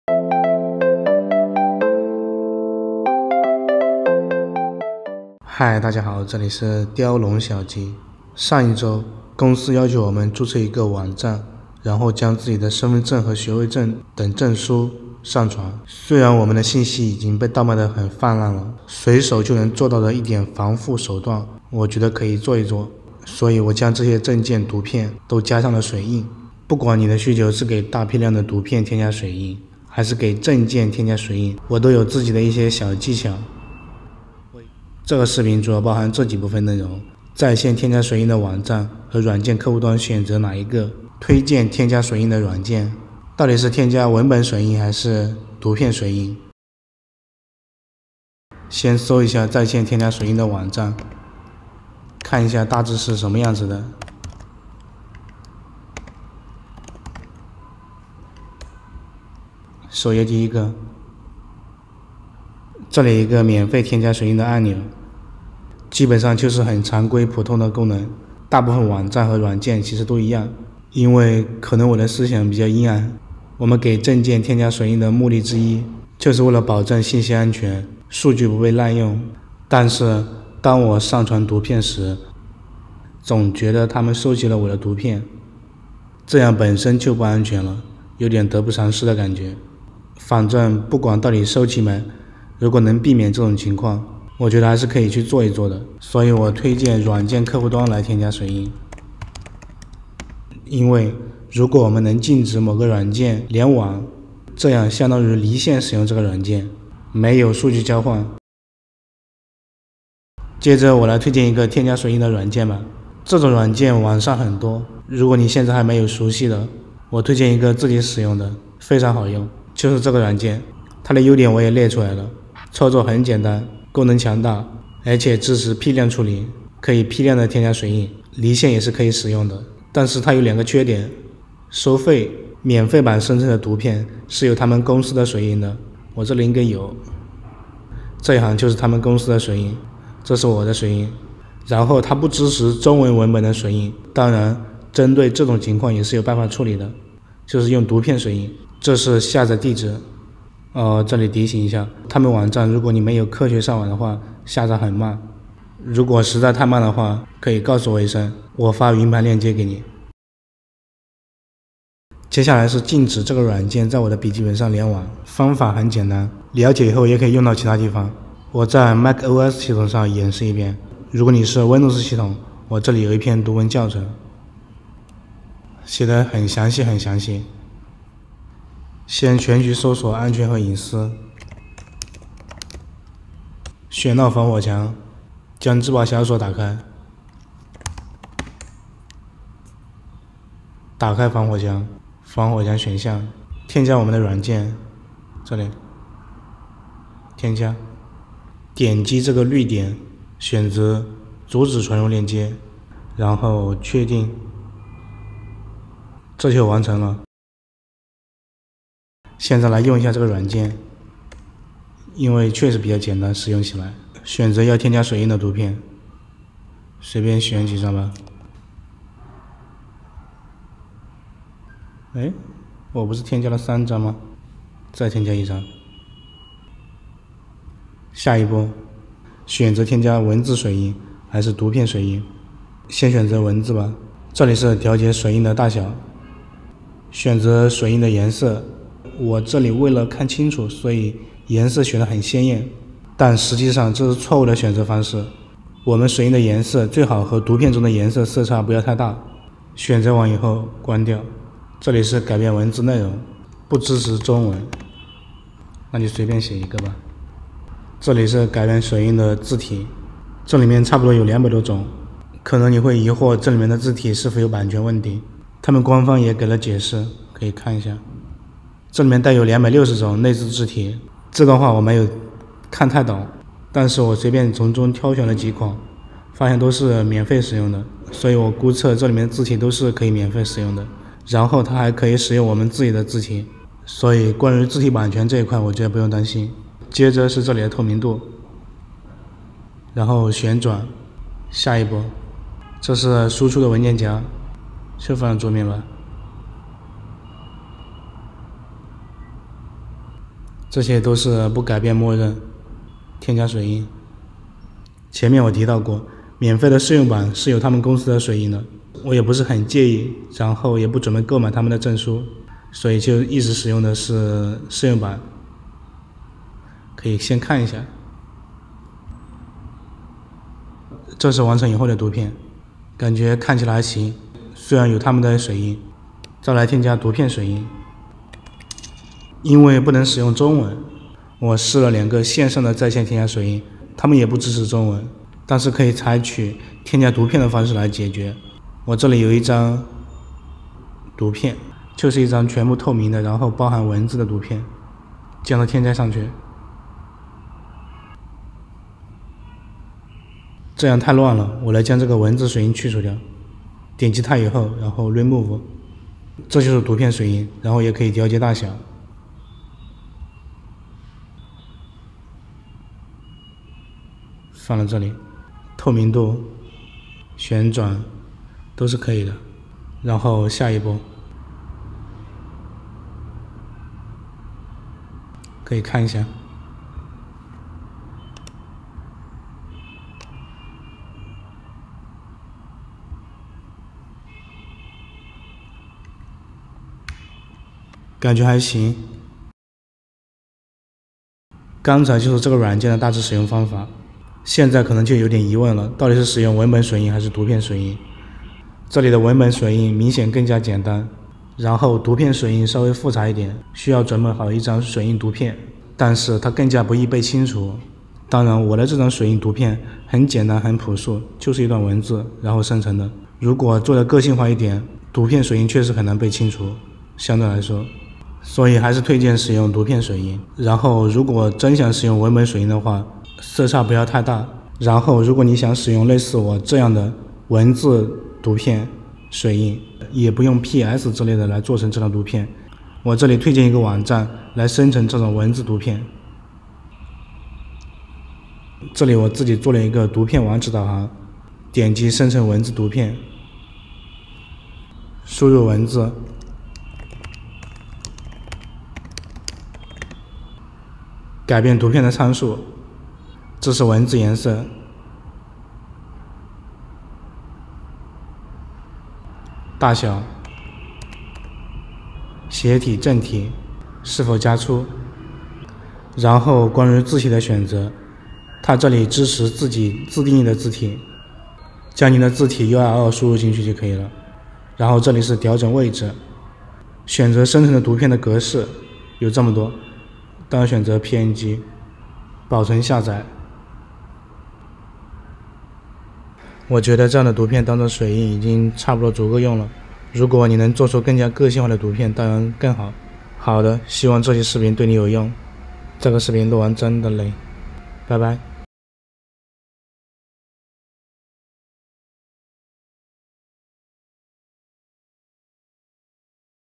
图片/证件照片加水印，禁止联网，以防监守自盗。批量处理。一点点隐私保护还是有必要，值得花费8 分钟观看
1、在线添加水印网站和软件客户端选择哪个？
不推荐在线网站 online
推荐软件客户端添加水印

2、批量添加水印软件
Visual Watermark
优点
操作简单，功能强大 。支持批量。
断网可使用，禁止网络数据交换
缺点
收费，免费版有 logo，
不支持中文文本水印，可用图片
Mac 禁止应用软件联网

3、文本水印和图片水印的选择
文本水印色差不要太大
推荐图片水印，难识别，不容易被清除

图片由Pettycon在Pixabay上发布
By PICOL in Picol Vector